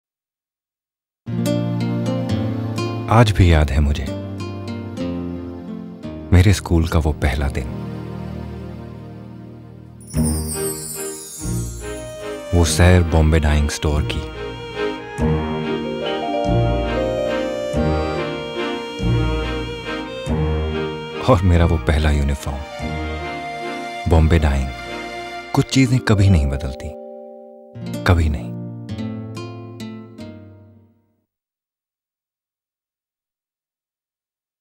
Bombay dyeing School Uniform TVC
It's a TVC for Bombay Dyeing School Uniforms, written by Yogesh Chandekar & Manish Kosambia and directed by Zakir Chinde of Cult Films. The Agency Handling the Account was St.Lukes Mumbai now known as 'Law & Kenneth'.